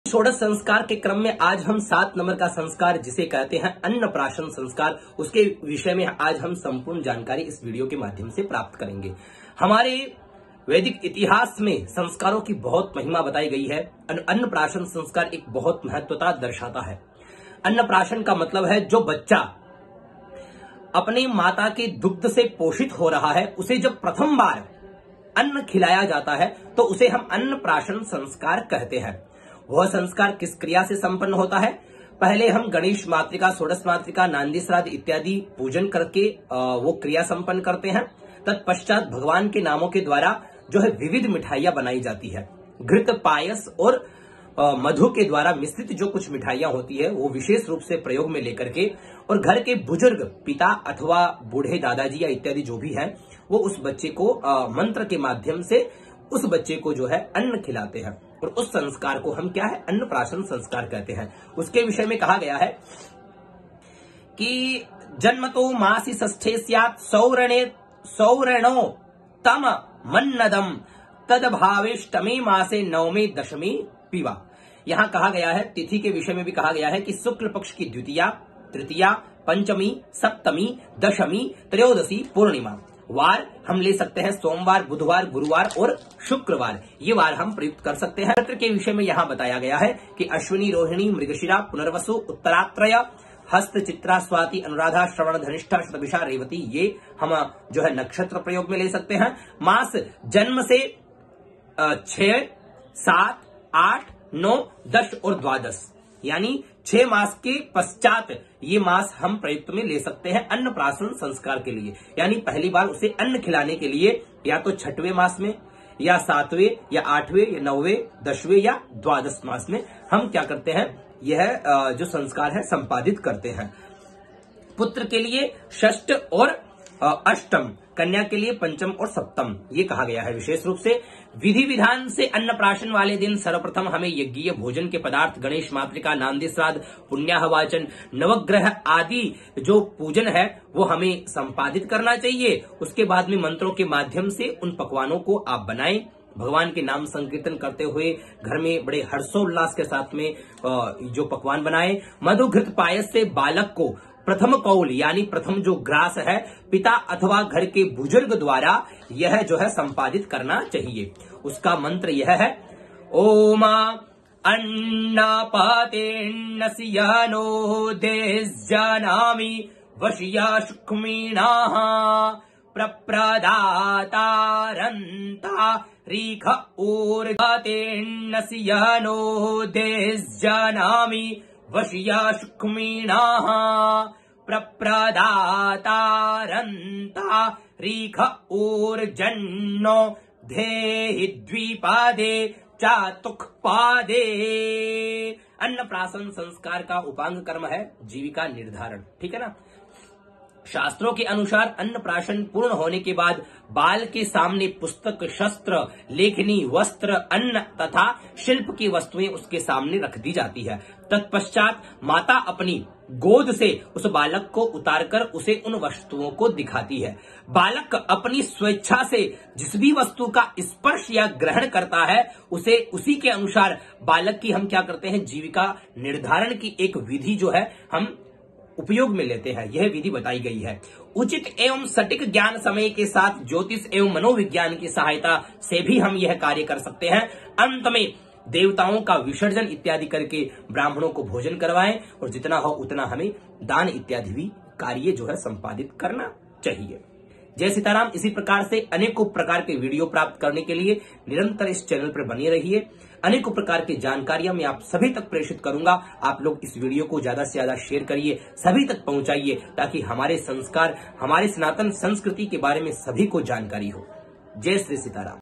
16 संस्कार, सप्तम संस्कार || 16 sanskar, 7 sanskar
16 संस्कार सीरीज में आज समाये है 'सप्तम संस्कार' का तो इसकी सम्पूर्ण जानकारी इस वीडियो में दी है।